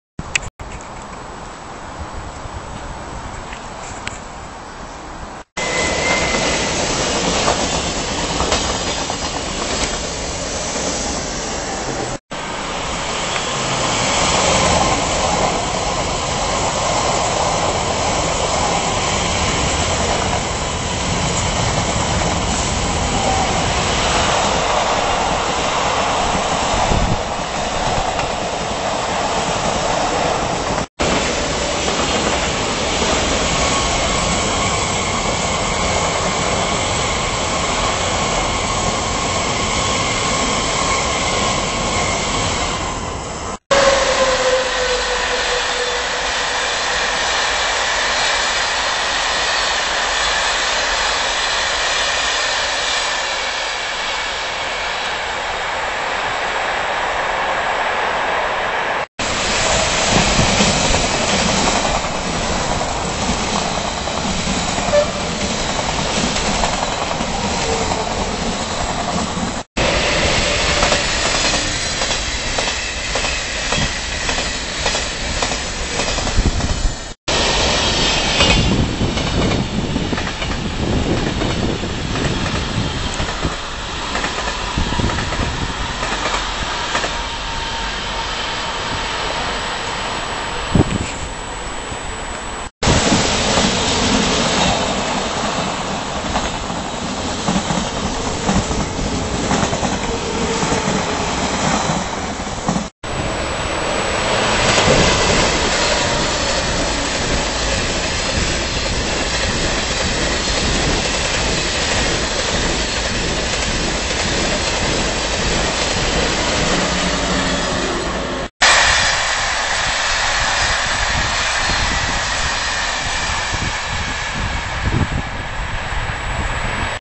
Tranis at Stade de France Saint Denis 31 January 2012
The video shows RER, SNCF, Thalys and Eurostar trains passing through Stade de France station in northern Paris during the very cold,grey late afternoon of tuesday 31 January 2012.